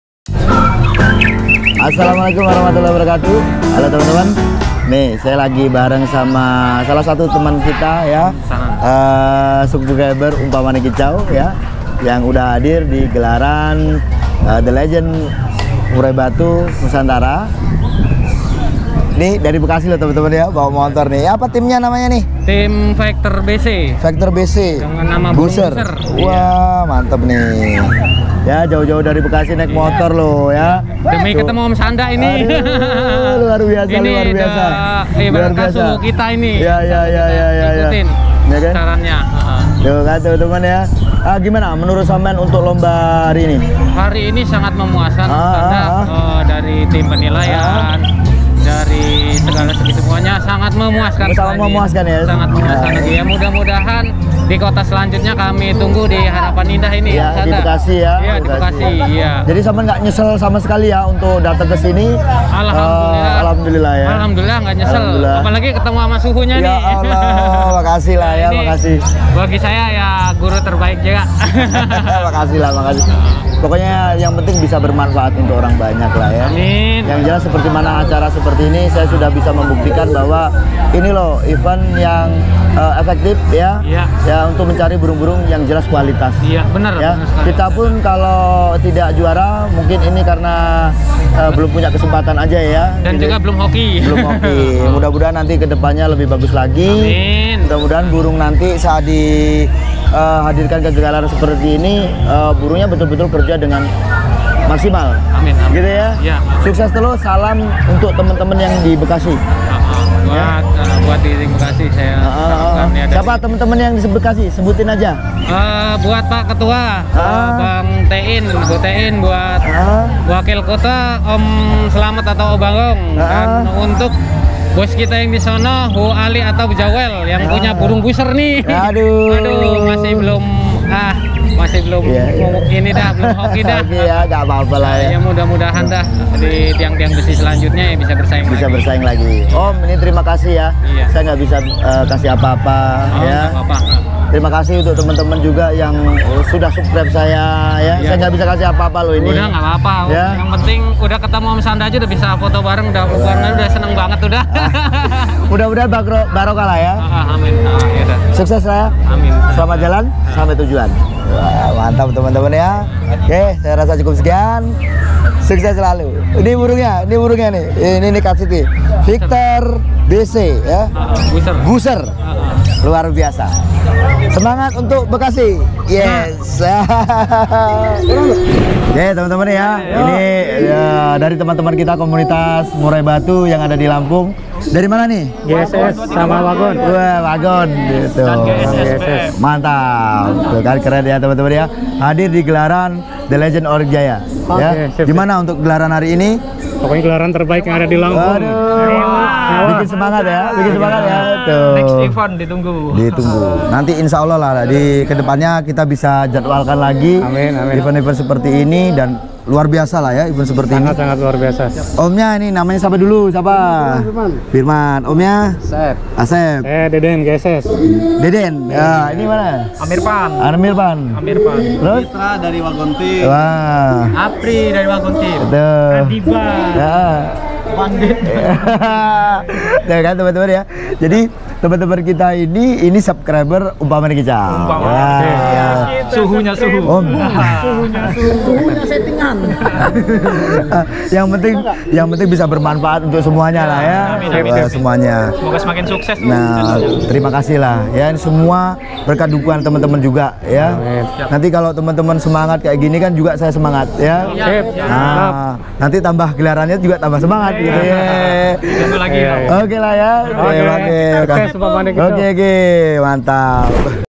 nekat naik motor dari bekasi ke Lampung cuma mau lomba burung
terimakasih untuk sahabat umpamane kicau bekasi.
loyalitas tanpa batas dari bekasi ke Lampung untuk mensukseskan gelaran the legen murai batu oriq jaya